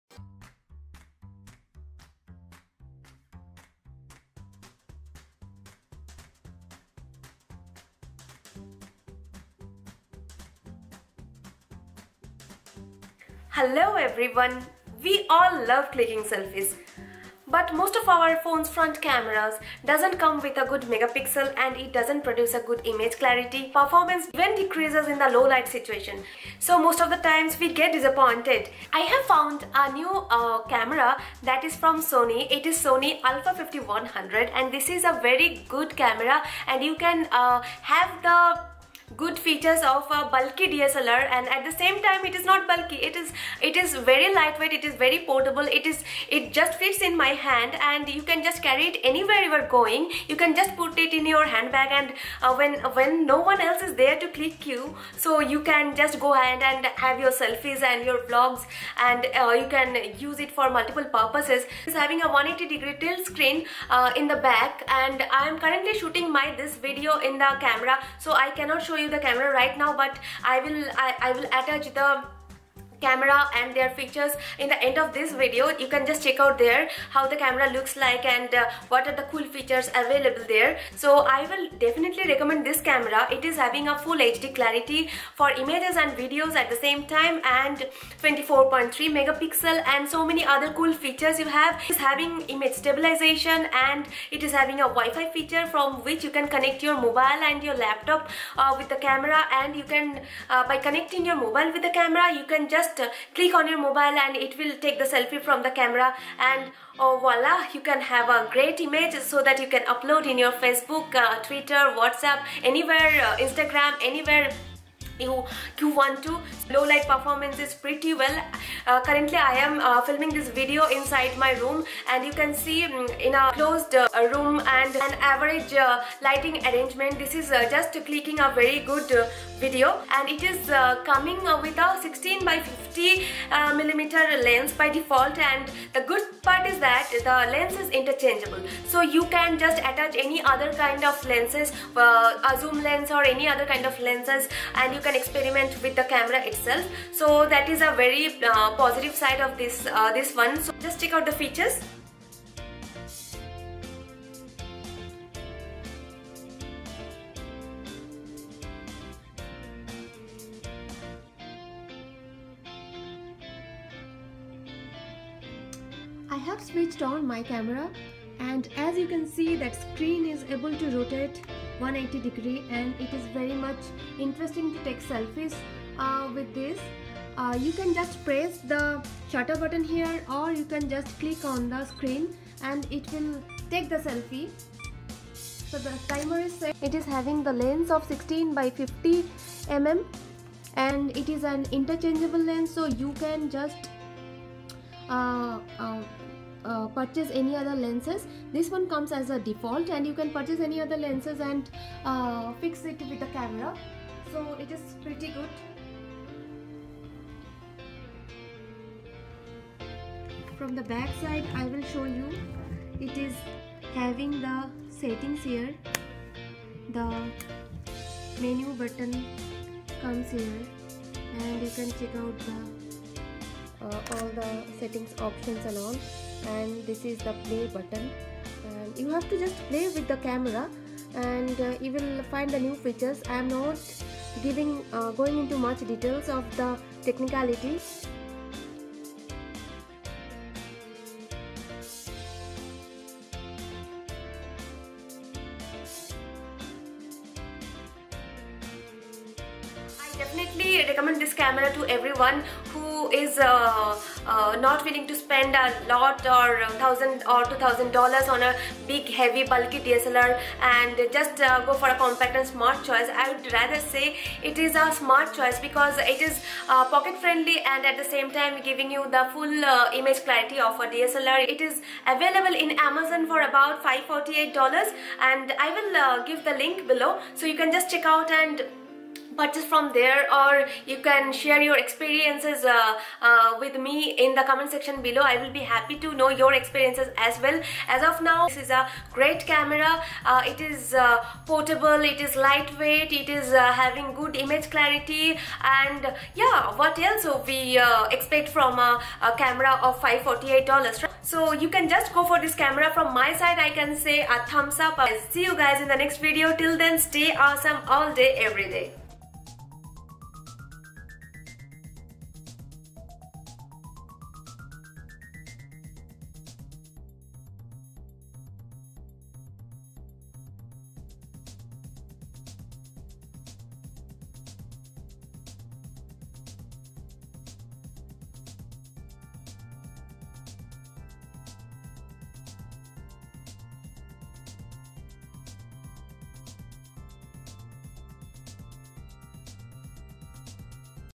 Affordable Vlogging Selfie Camera 4K Video || SONY a5100 || First Look n Footage
Hello Everyone.. Sharing with you my new Favourite Selfie Camera.
Say Good Bye to the dull Front Camera Pics. Because YOU are SPECIAL!!

You can find the camera in amazon